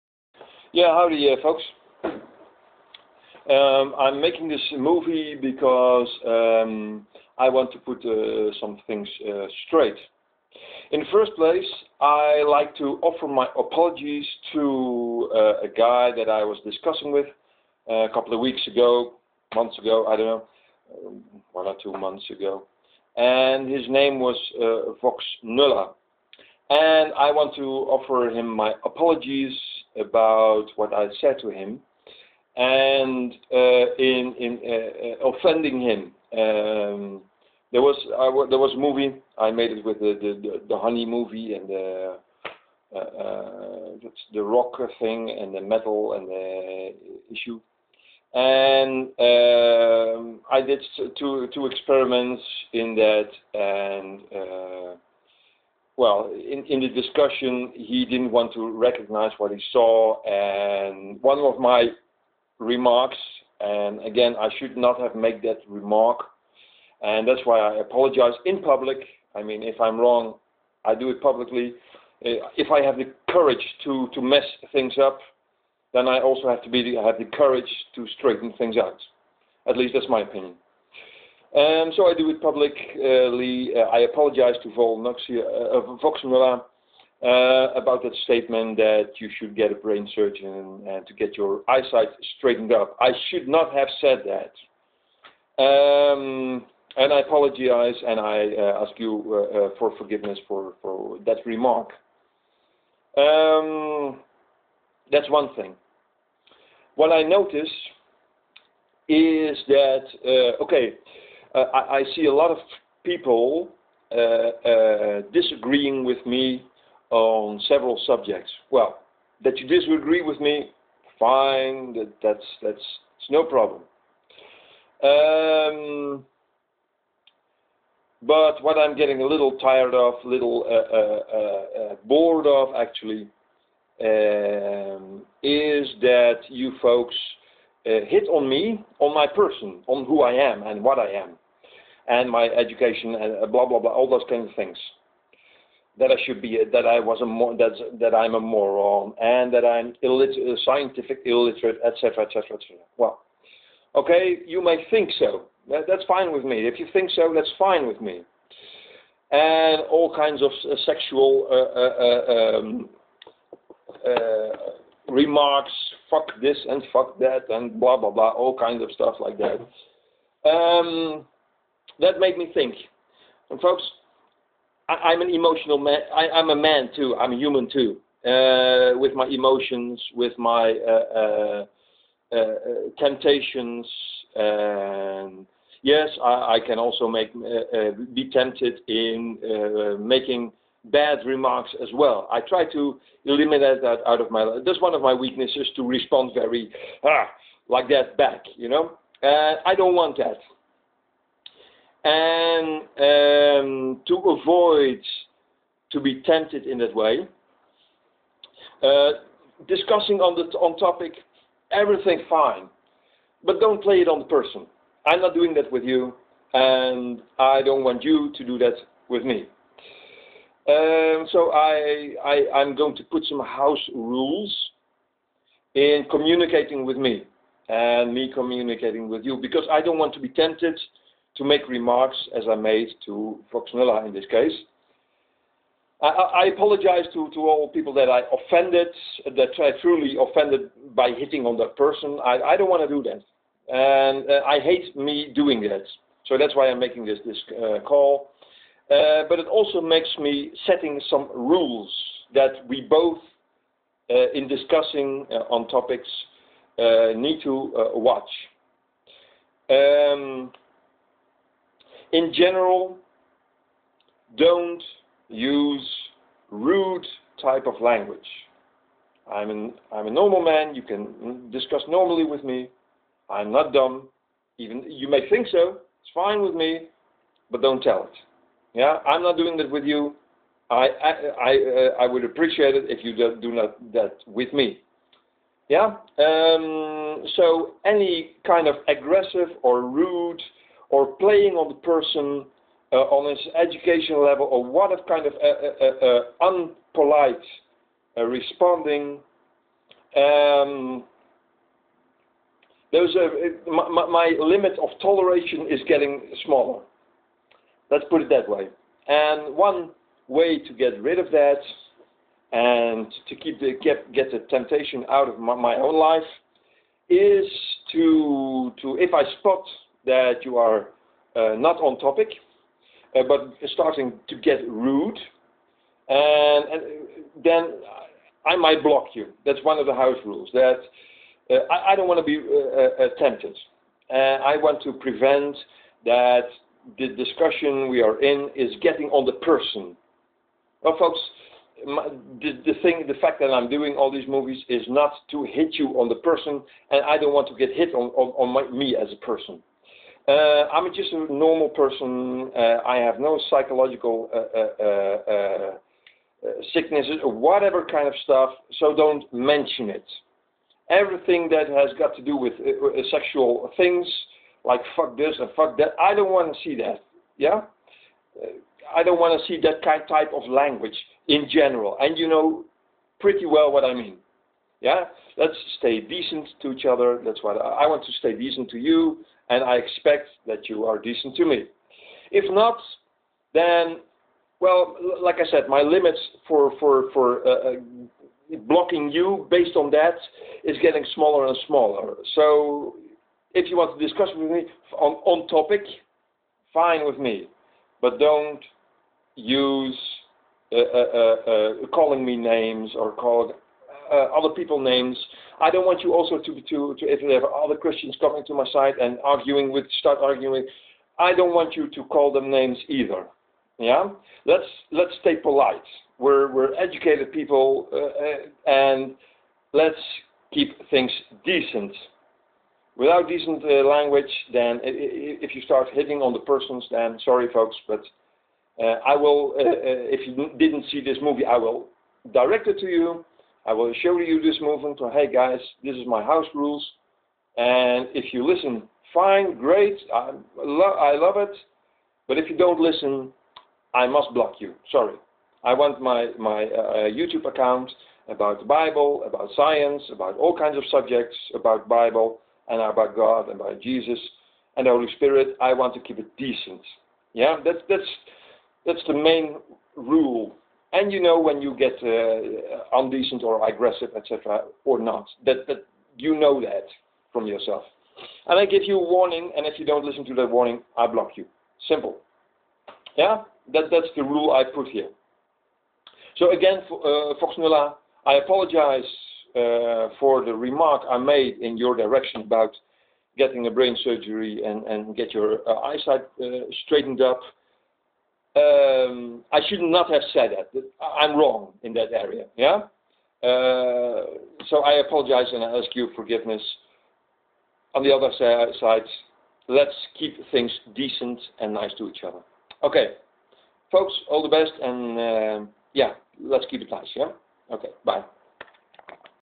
My public appologies to Voxnulla and my YT house rules
I had a discussion with a guy with a youtube name voxnulla and i told him some rude stuff: hereby I publically appologize to him voxnulla about my rude remarks. and i ask his forgiveness about this.

Second thing is: lots of people hit on me on a unpolite/undecentand mainly negative way. And that made me think: I dont want that. i dont want to be treated/addressed rudely in your comments and I dont want to be rude back to you and i dont want even to be tempted to get rude to you folks.
So based on that i have a simple house rule to you folks:
I have statrted this outube account to get discussions with you about the toppics i bring. It is super if we get ontopic of that thats fine. But i dont appreciate that we get personal to eachother in remarks about eachothers person. You may not like me thats fine but dont spit that out in your remarks keep your opinions about me as a person to yourself; you may disargee with me but dont get personal or agressive or unpolite... I am here on YT to decently talk to you folks.

i dont like to see any kind of  rude types of language on my YT account..

If you do you will get 1-2 warnings and i show you this movie (i will sent it to you) and then it is over; i will block you if you dont listen. i want to keep things nice between you and me.